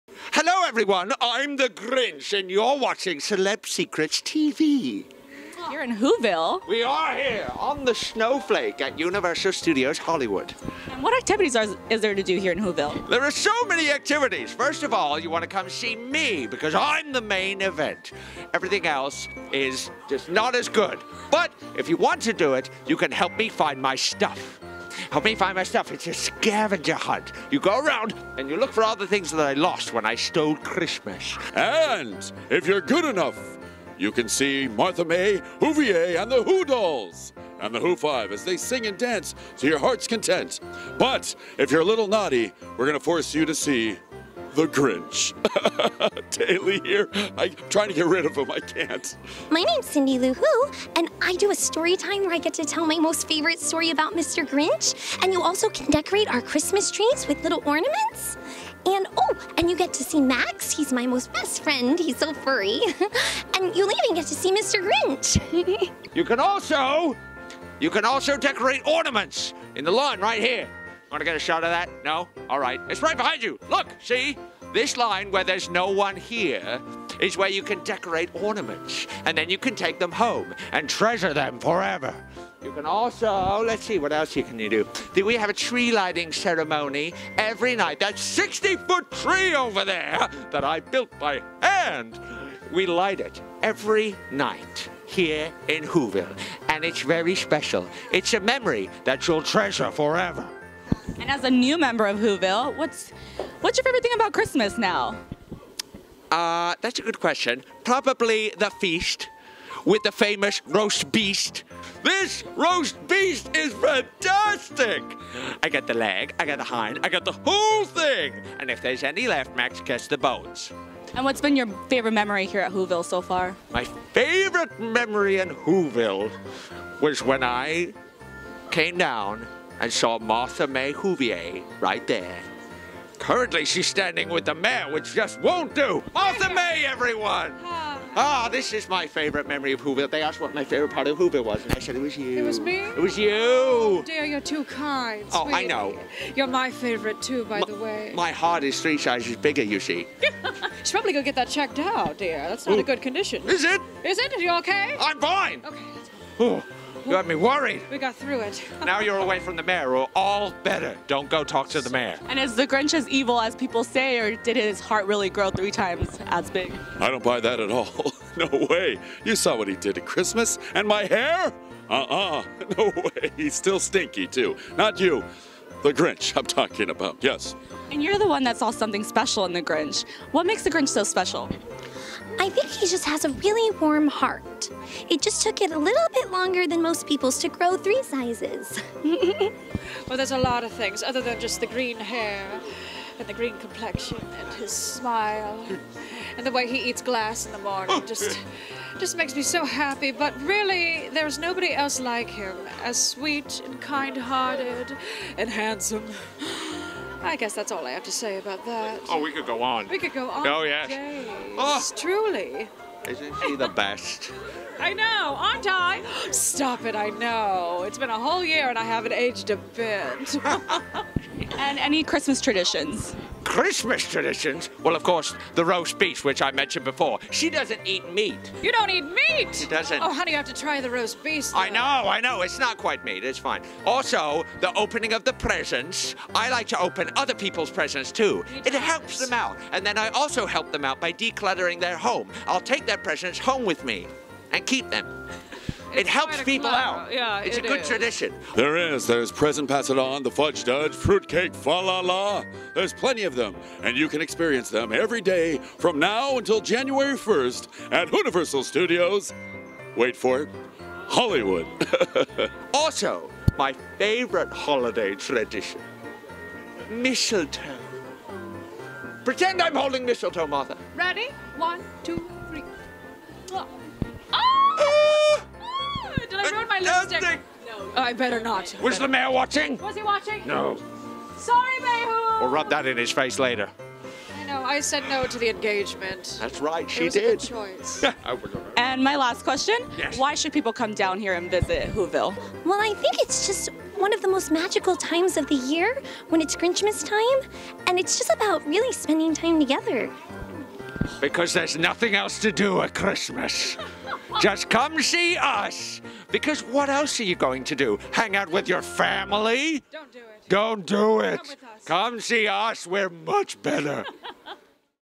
Our Day at Grinchmas at Universal Studios Hollywood with THE GRINCH!
Celeb Secrets heads to Who-ville for the day to talk to The Grinch, Cindy-Lou Who, Martha May Who-vier, and the Mayor of Who-ville at Universal Studios Hollywood!